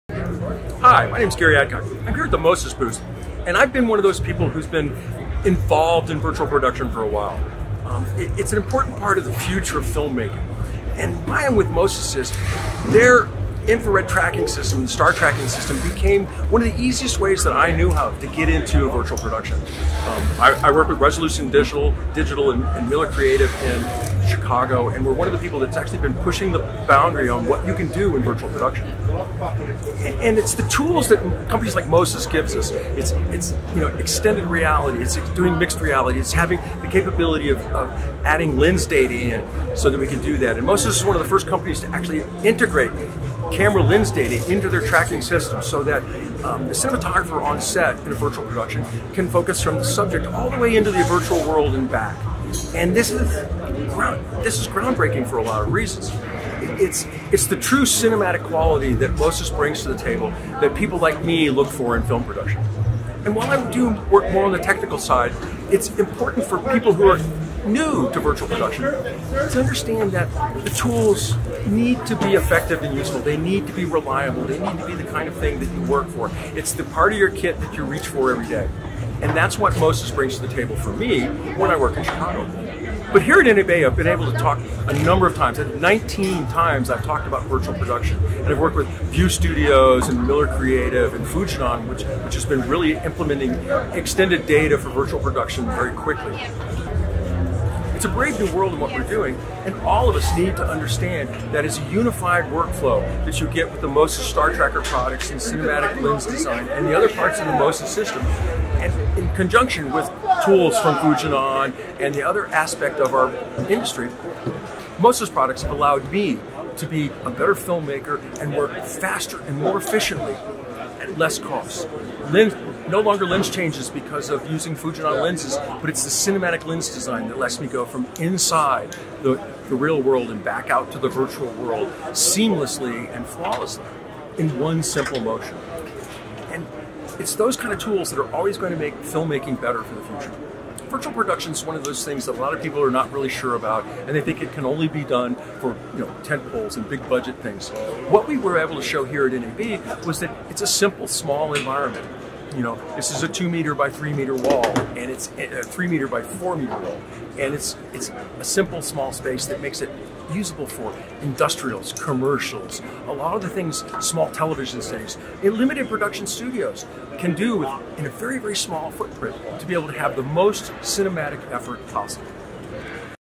Gary Adcock - Virtual Production with Mo-Sys - NAB 2022
"It's the true cinematic quality that Mo-Sys brings to the table that people like me look for in film production" - Gary Adcock talks about Virtual Production with Mo-Sys at nabshow2022.